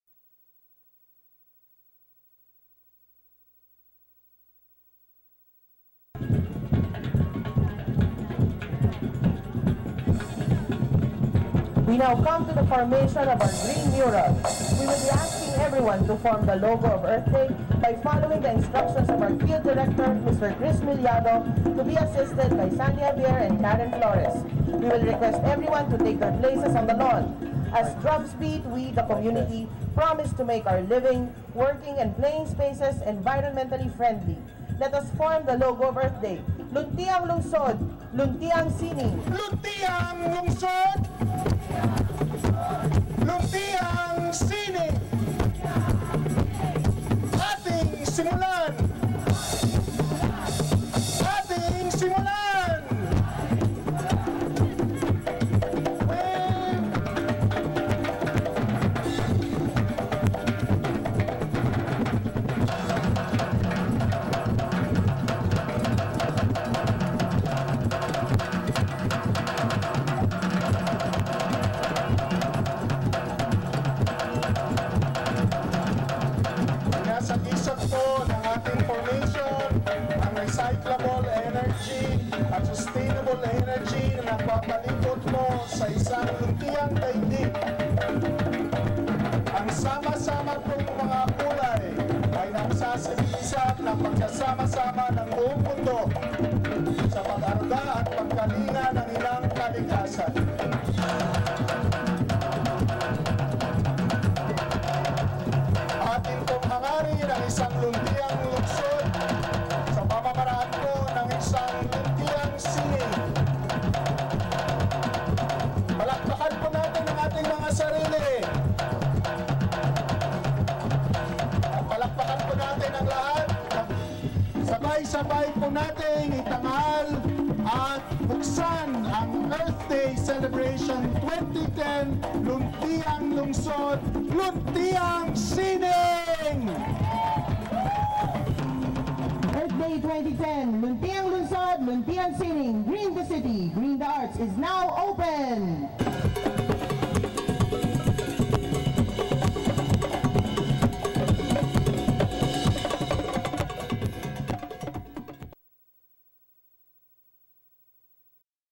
Human Mural @ CCP Earth Day 2010 Celebrations
Luntiang Lungsod Luntiang Sining
Earth Day 2010 Celebration at the Cultural Center of the Philippines

A Human Mural featuring the Earth Day logo! Participants from CCP Employees, CCP Resident Artists and participating Environentalist Groups